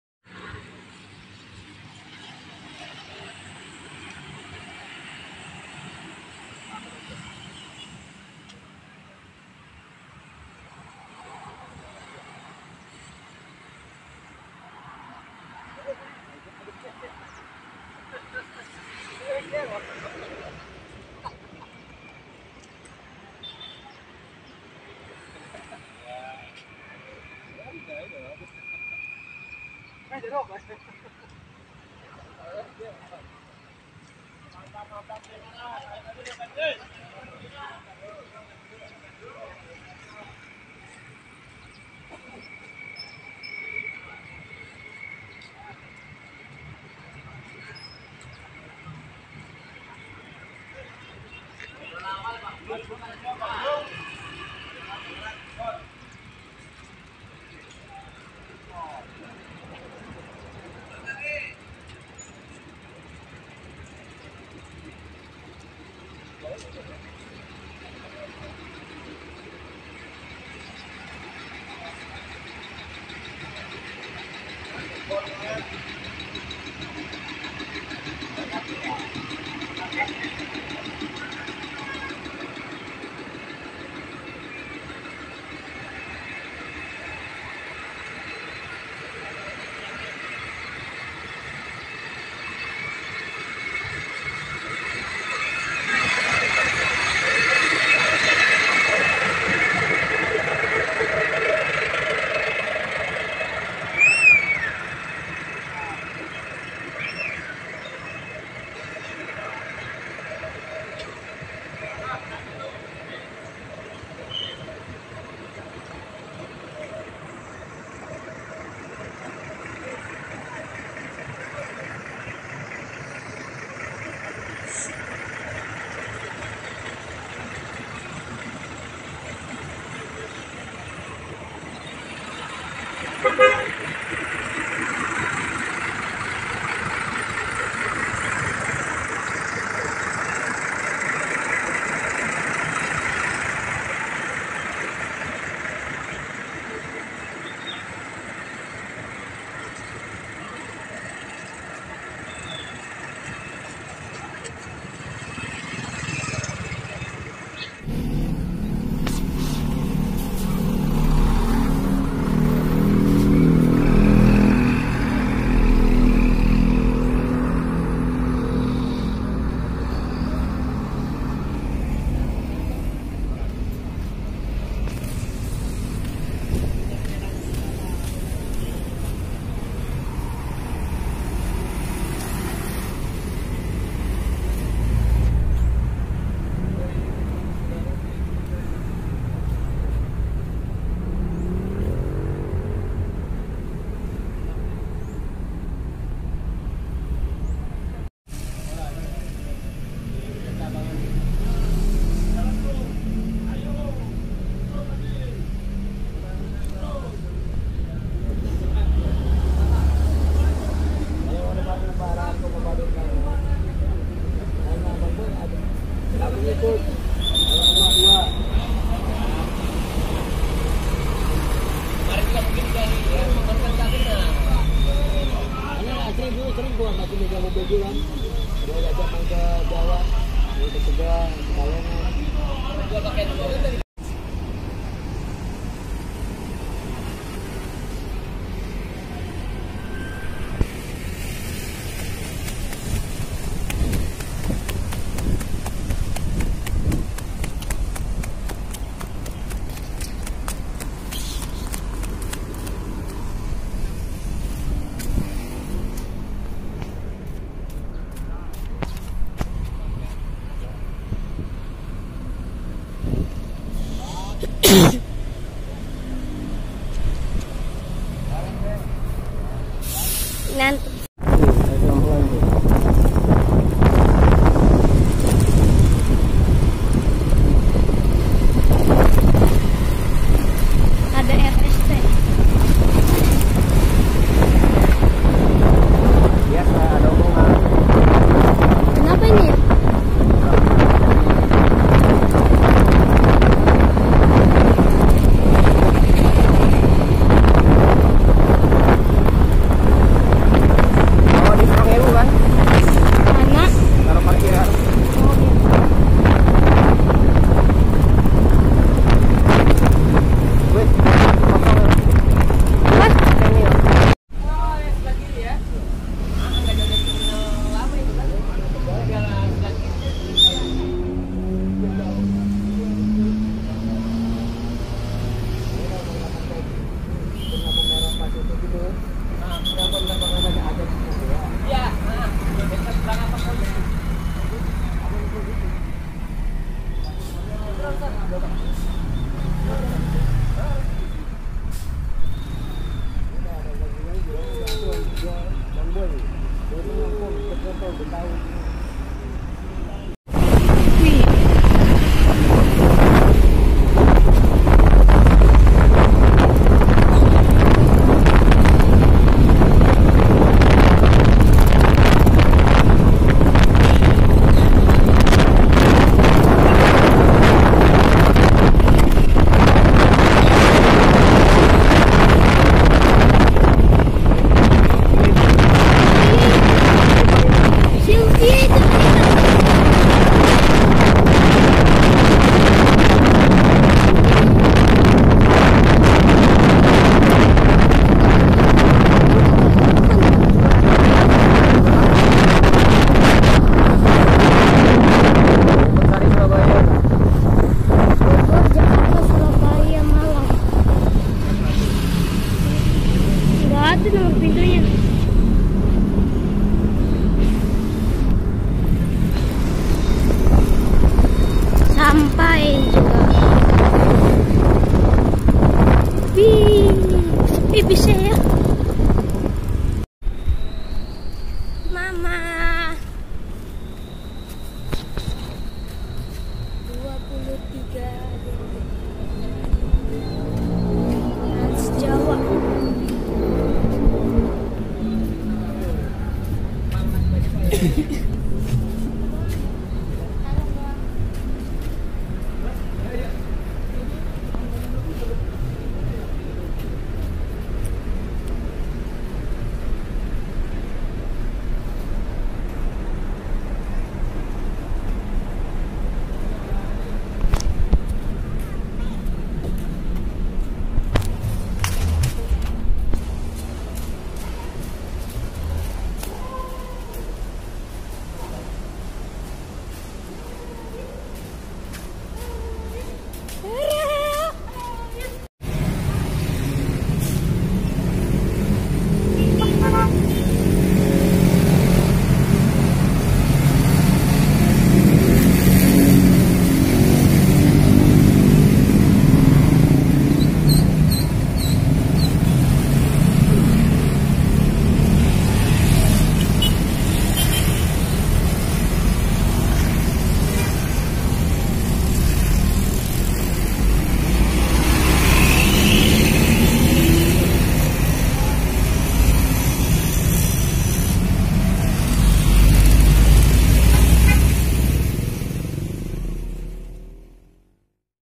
Pool Bus Sinar Jaya Cibitung dan Bus Di Terminal Dukuh Salam Slawi
pool bus sinar jaya Cibitung Bekasi Jawa barat, hiruk pikuk keluar masuk bus sinar jaya berbagai jurusan